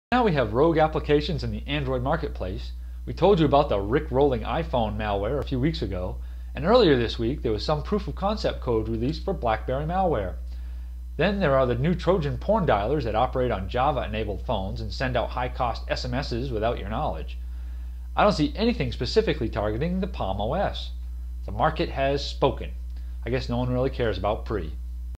HNNClip011510-A5: Mobile Trends
So now we have rogue applications in the Android Marketplace, we told you about the rick rolling iPhone malware a few weeks ago, and earlier this week there was some proof of concept code released for Blackberry malware. Then there are the new Trojan porn dialers that operate on Java enabled phones and send out high cost SMSs without your knowledge. I don't see anything specifically targeting the Palm OS. The market has spoken, I guess no one really cares about Pre.